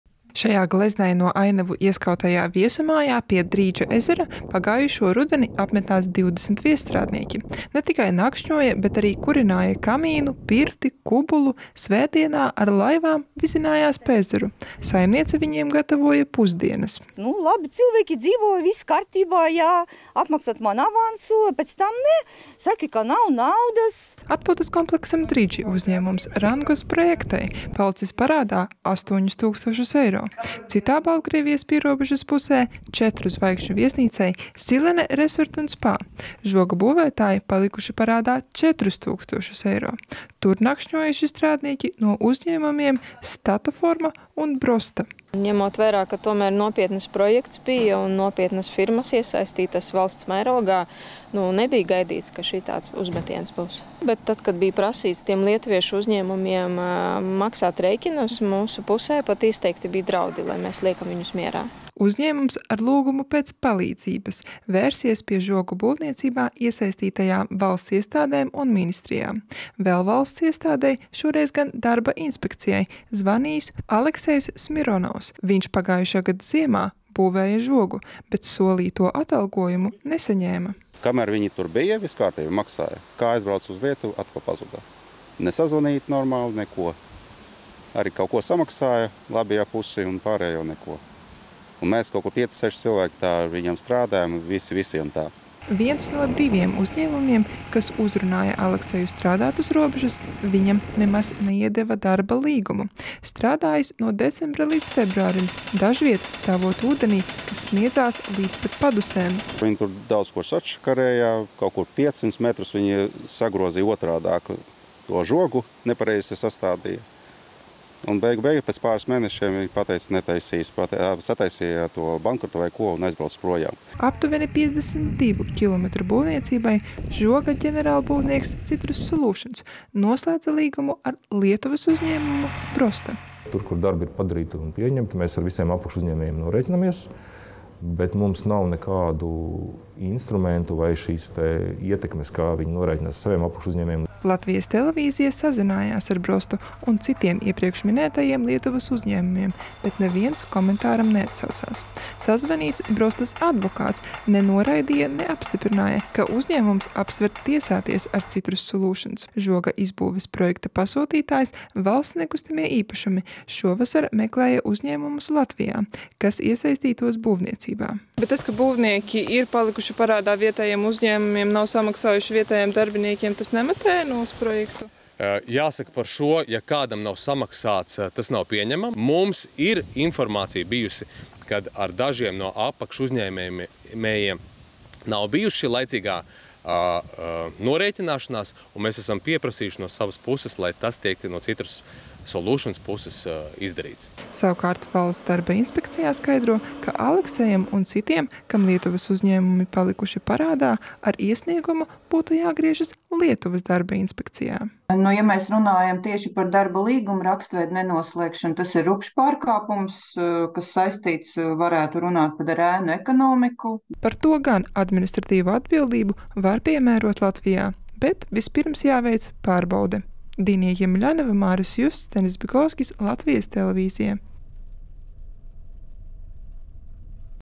Žoga būvnieki palikuši parādā viesu namiem un strādniekiem
Kamēr politiķi un eksperti spriež – cik ātri paspēsim uzcelt žogu uz austrumu robežas, viesu namu īpašnieki Baltkrievijas pierobežā jau vismaz kā pusgadu gaida apmaksu no žoga būvniekiem. Vairākas Lietuvas firmas palikušas parādā par darbinieku izguldīšanu un barošanu. Tāpat arī solīto atalgojumu par darbu ziemā purvainās teritorijās nav saņēmuši strādnieki. Ģenerāluzņēmējs “Citrus Solutions” apgalvo, ka apakšuzņēmējiem nav palicis parādā.